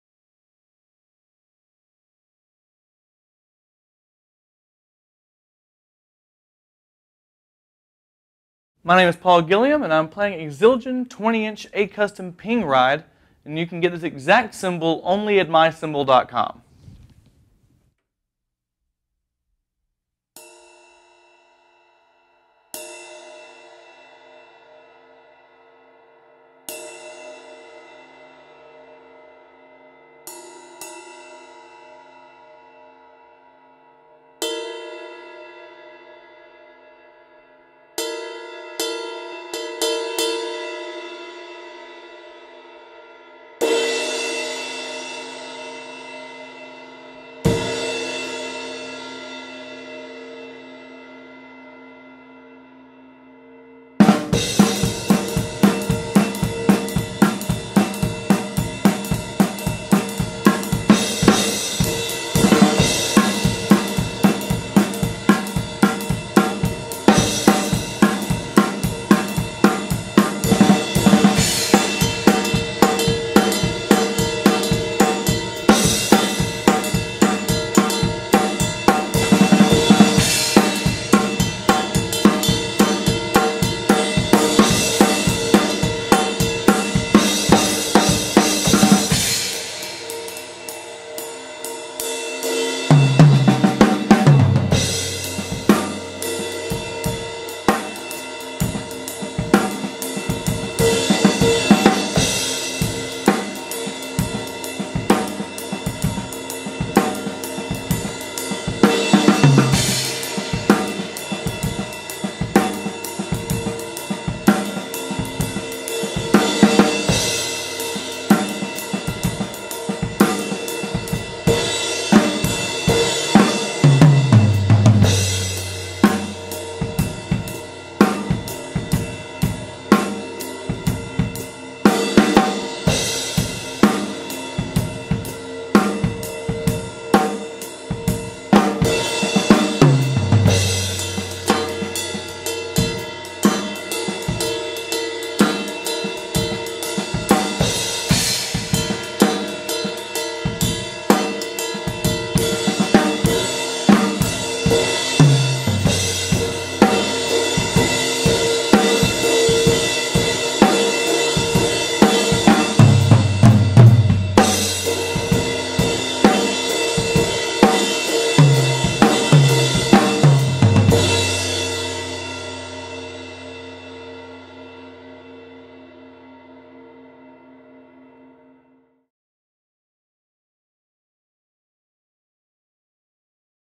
Zildjian 20" A Custom Ping Ride Cymbal (A20522-1112013Q)
*SOLD* Zildjian 20" A Custom Ping Ride Cymbal from Memphis Drum Shop. See more Zildjian 20" A Custom Ping Ride Cymbals

Other gear used in this video:
• Cymbals (From drummers L to R):  Zildjian 13" A Custom Mastersound Hi Hat Cymbals, Zildjian 14" A Custom EFX Cymbal, Zildjian 18" A Custom Fast Crash Cymbal